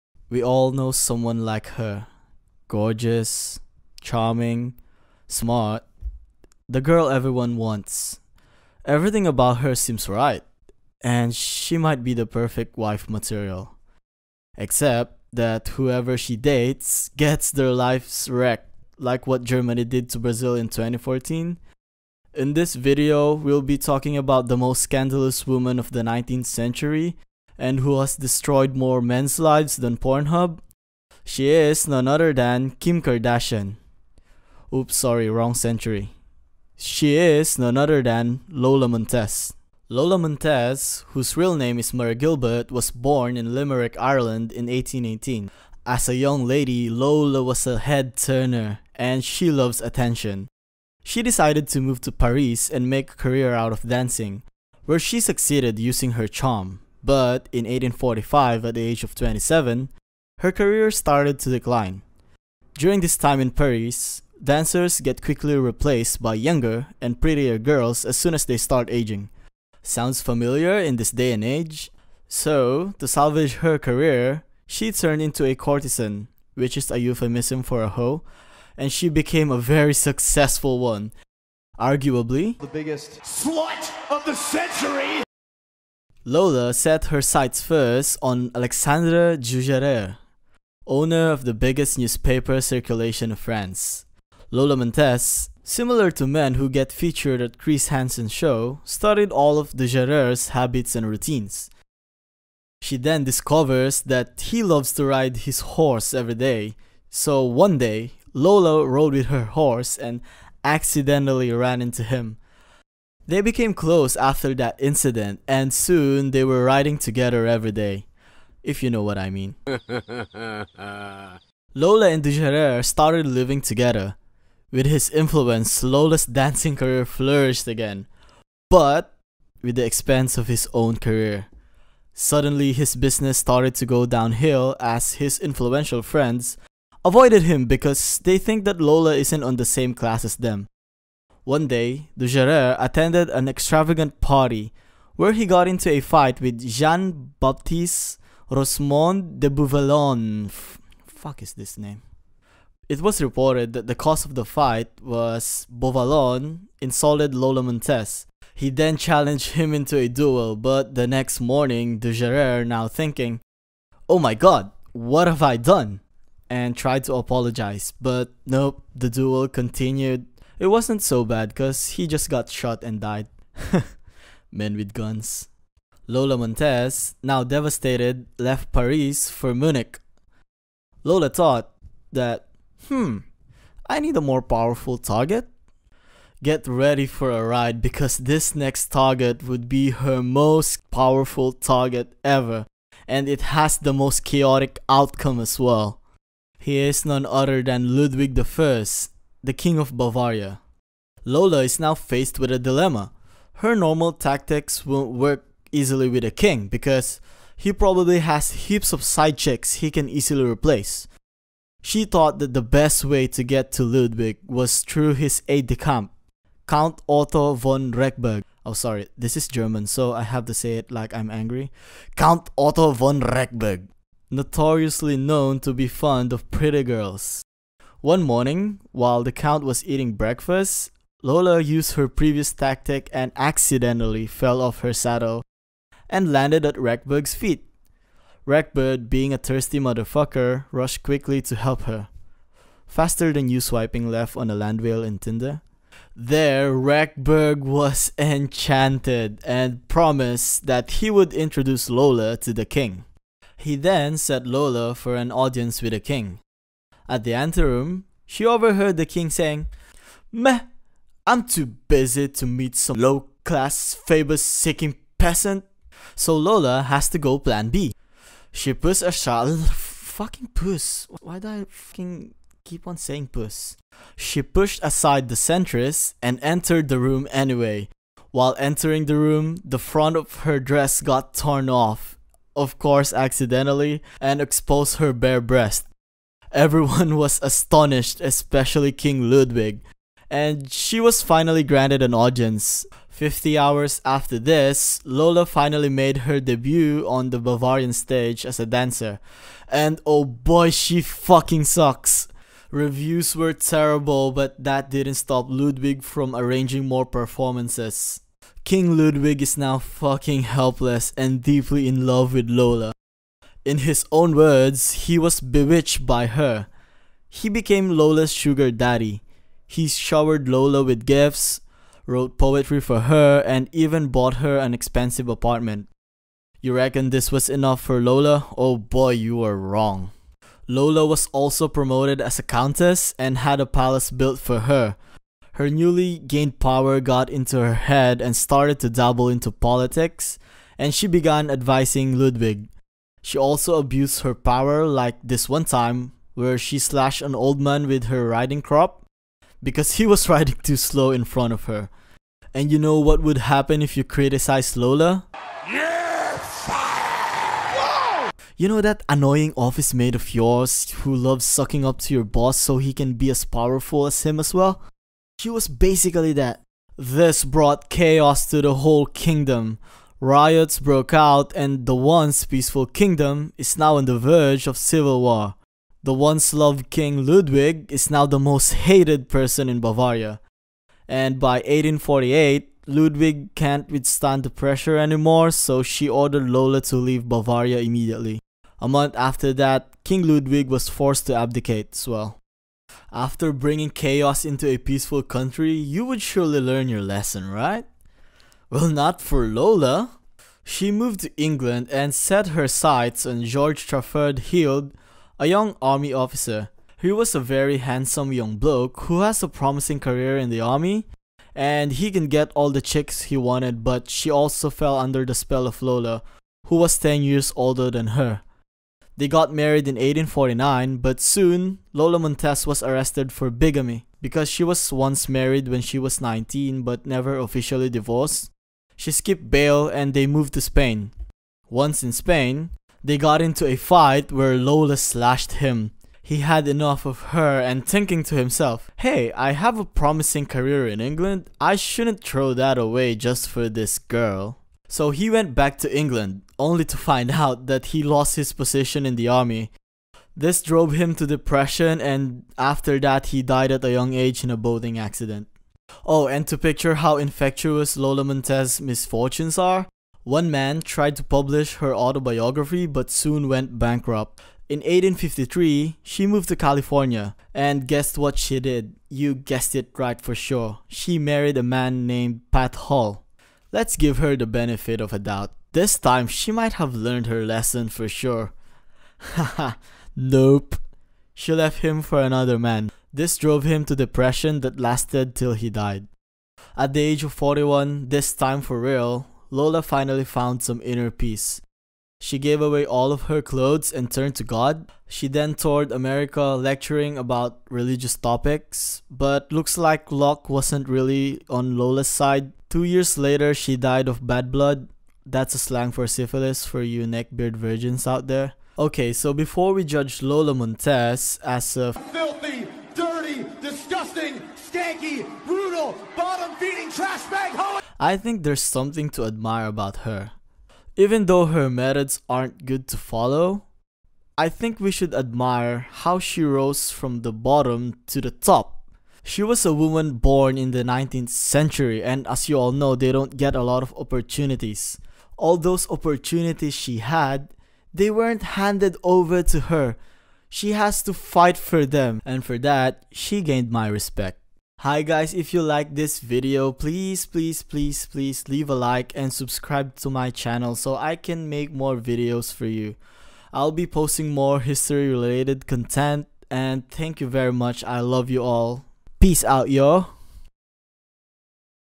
The Most Scandalous Woman of 19th century: Lola Montez
In this video we'll be talking about Lola Montez, who seduced Europe's most influential men of the 19th century. Find out how she has wrecked their lives!